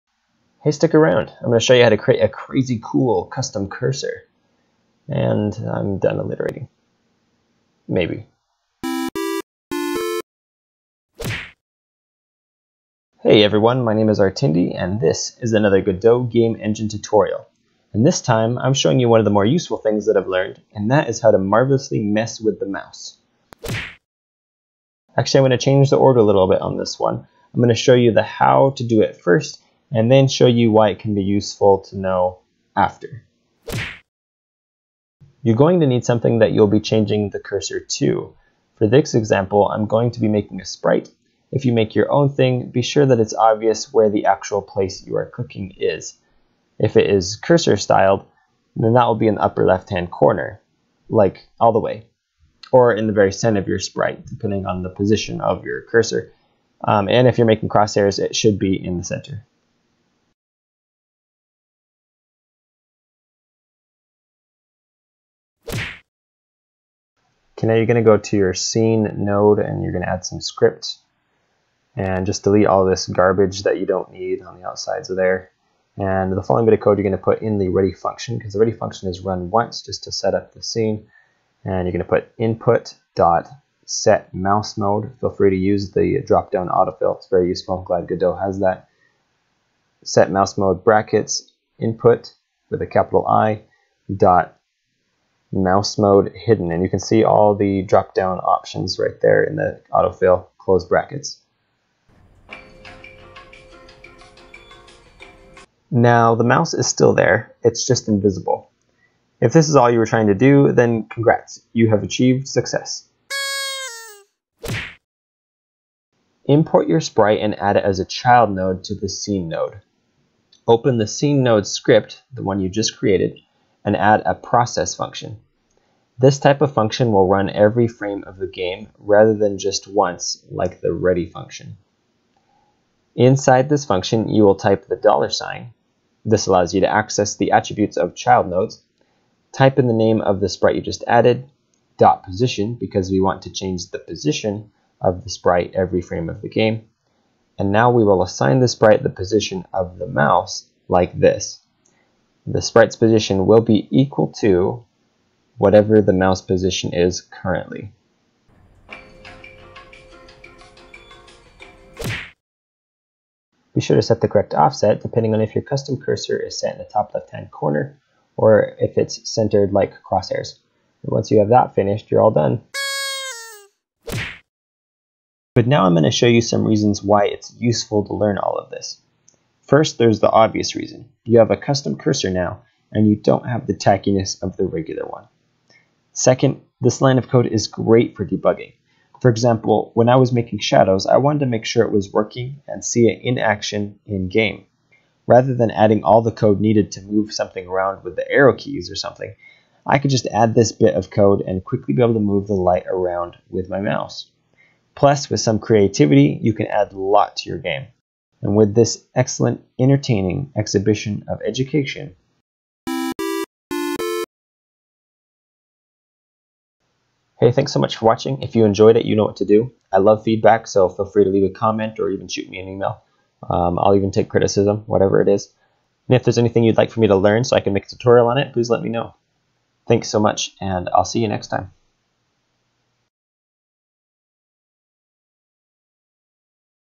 How to Make a Custom Cursor - Now You Know Too - Godot Tutorial
Why are tutorials often super boring? This week I'll show you how to add your own mouse cursors to your game and how it can help you better create and troubleshoot your game as well.